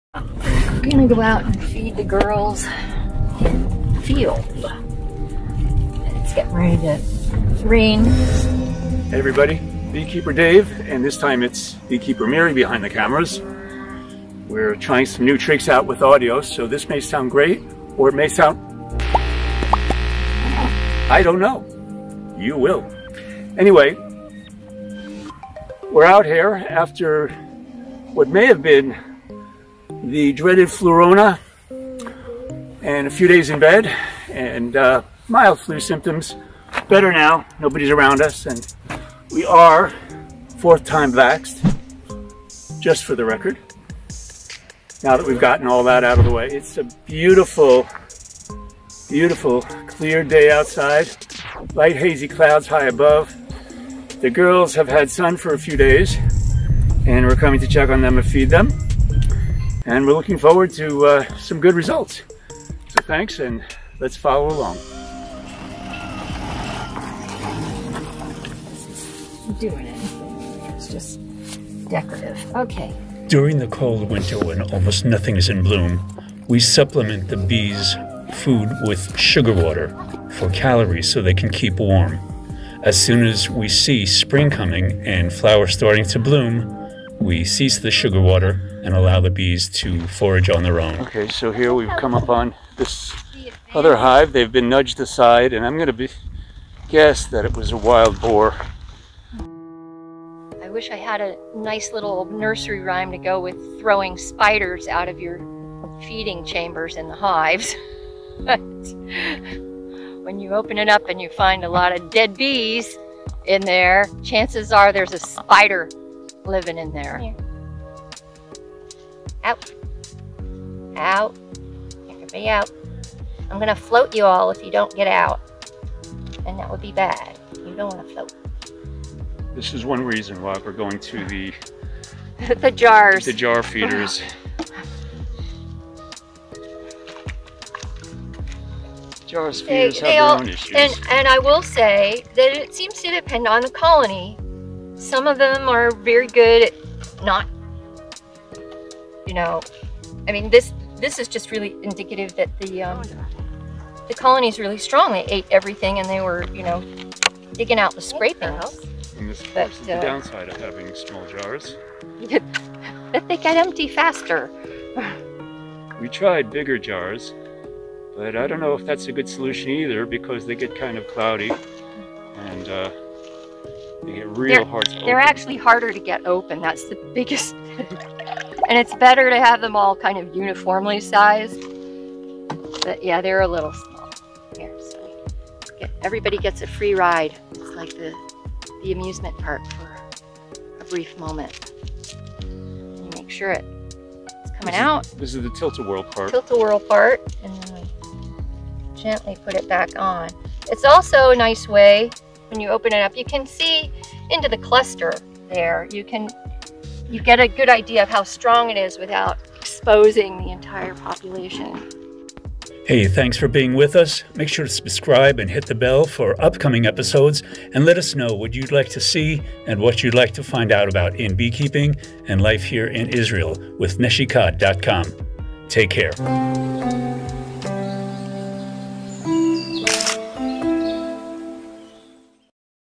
Winter Hive Feeding for Tu'B'Shvat
Neshikha's beekeepers Miri and Dave feed the hives on a fine winter's day, as Israel and the Jewish world celebrate - how fittingly - "Tu'B'Shvat" - the "New Year of the Trees" (think, roughly, of Arbor Day in the States but with far more, and deeper ecological and spiritual relevance for all of humanity): Our latest video. Enjoy

"Tu BiShvat is the Jewish New Year for fruit trees. It’s a day to celebrate the new fruits of the season and to remember our connection to the earth. But why do we celebrate a new year for fruits? How is Tu BiShvat still meaningful today?

"In modern times, Tu BiShvat has become a somewhat environmental holiday, incorporating planting trees and respecting nature. Interestingly, the calendar year is not simply a function of Rosh Hashanah, the most famous Jewish New Year."

Shot on a Galaxy S20Ultra and Edited in KineMaster.